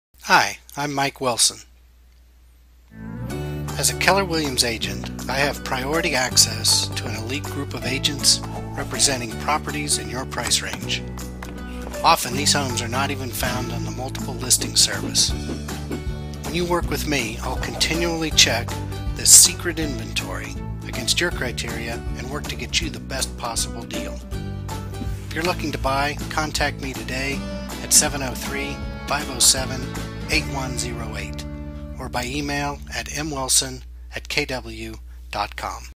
Secret Inventory - Homebuyers Get Access Here
Work with an experienced, well-connected, agent and get access to the 'secret inventory'.  It's a big part of why some buyers searching for homes for sale in Loudoun are more successful!

Author: Michael Wilson
Loudoun Real Estate
Keller Williams Realty - Ashburn VA